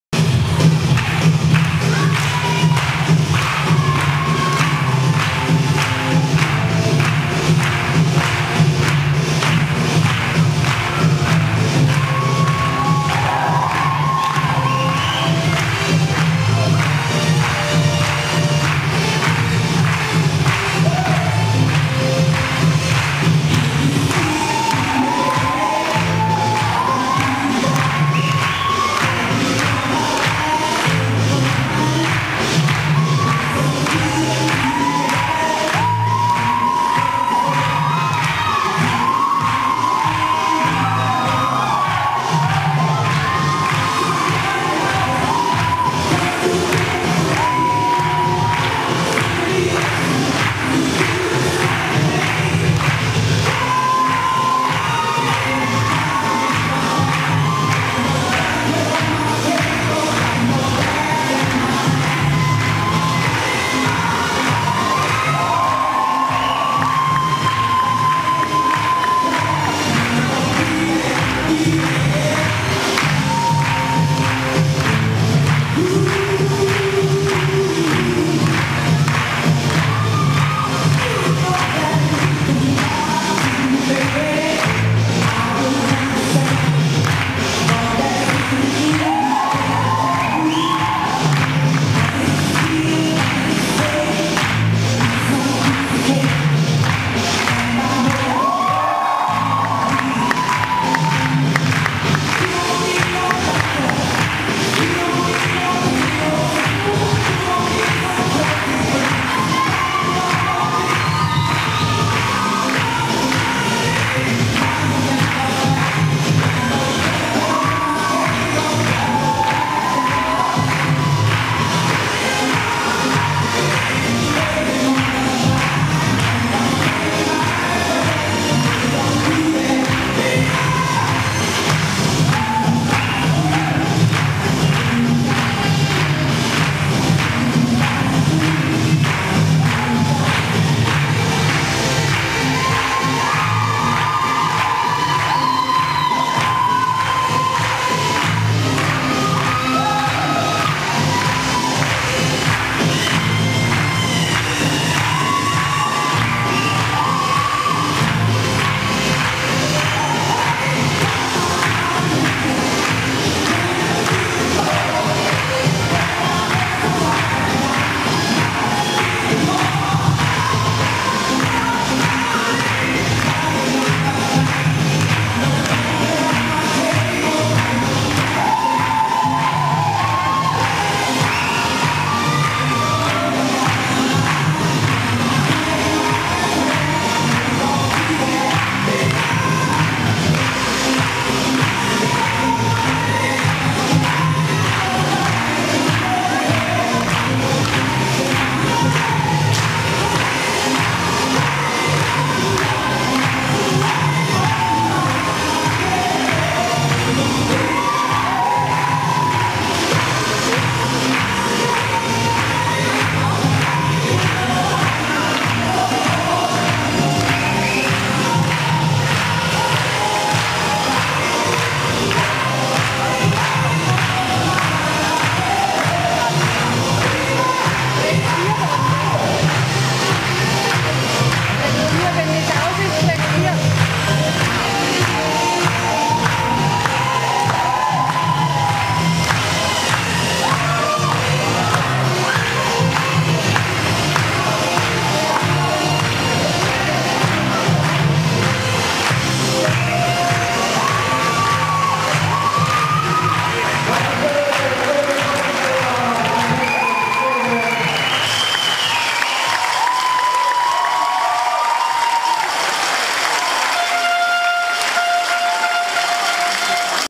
Showauftritt "Anzfelden" Bad Liebenzell
Showauftritt der Eyecatchers (SV Anzing) und Rocking Baloos (1. Eggenfeldner Rock'n'Roll Club '78 e. V.) in Bad Liebenzell 2011.